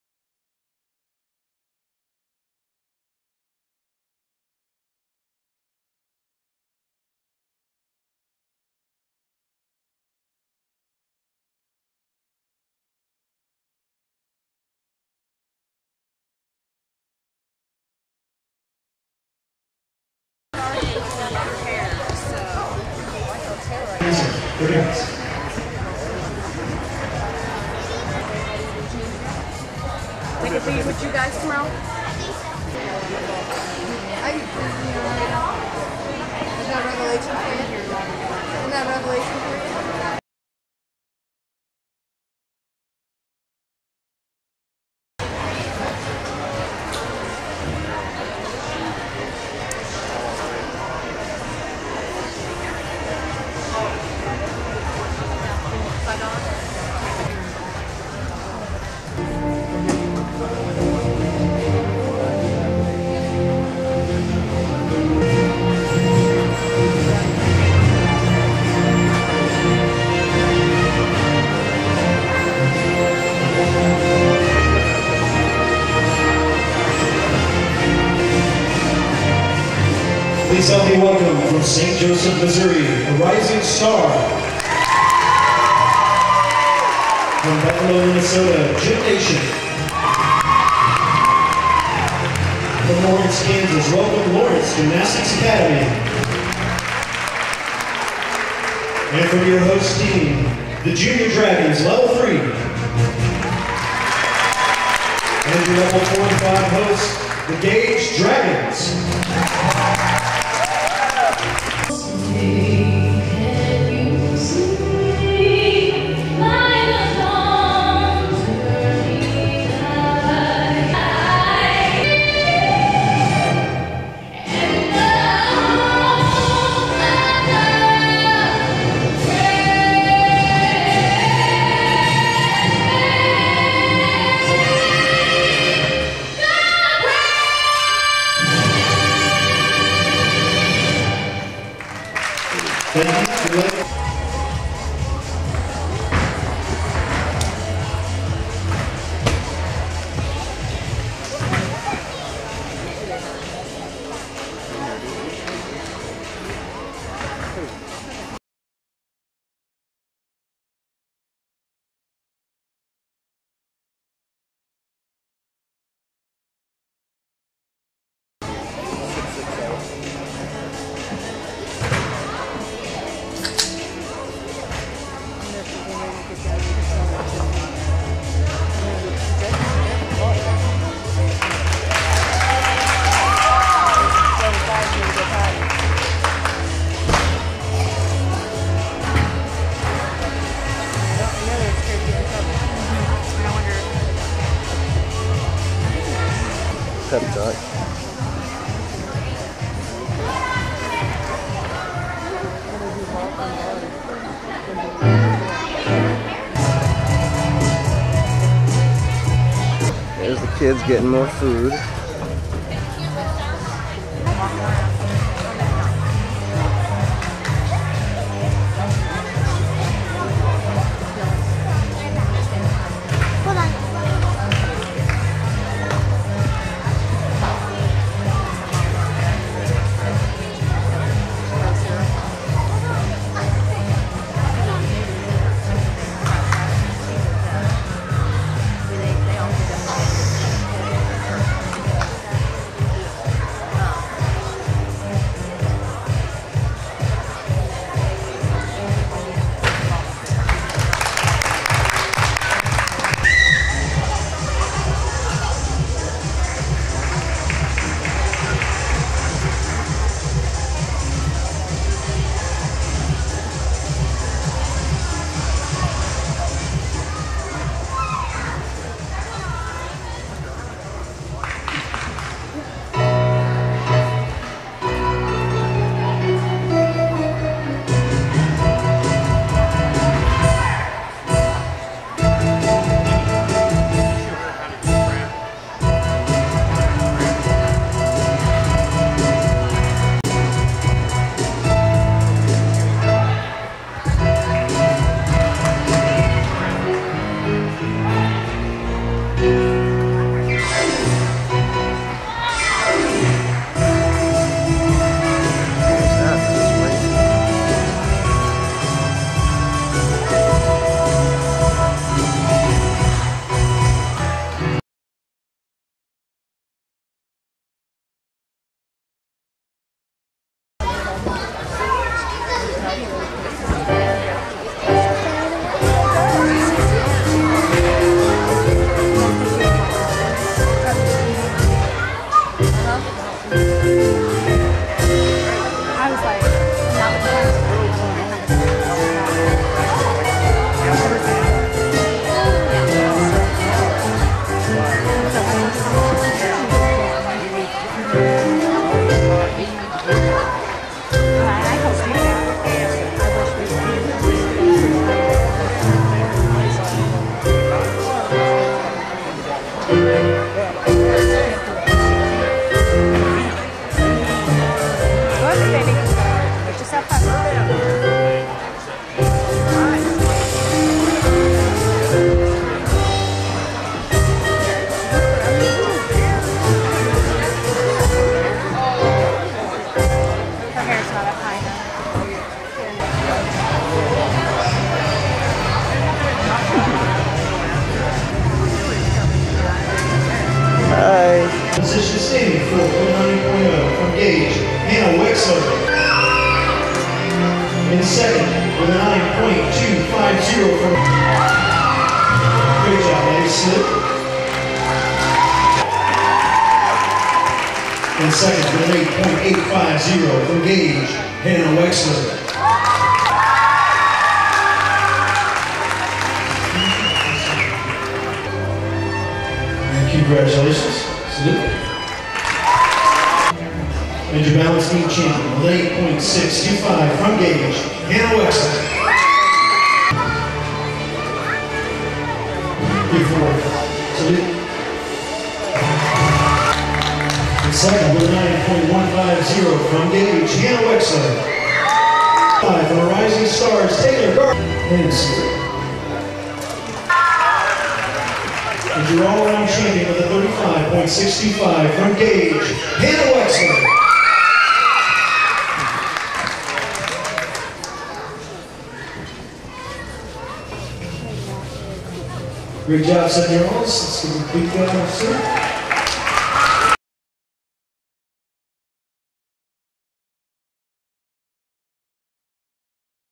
2010 Dragon Invitational - Hannah Wexler.mpg
Hannah took the All-Around Title at the 2010 Dragon Invitational for the USAG Level 4 - 7 year old division at the GAGE Center.  This is Hannah's 3rd meet in a row taking the all-around for her level and age group!  Go Hannah!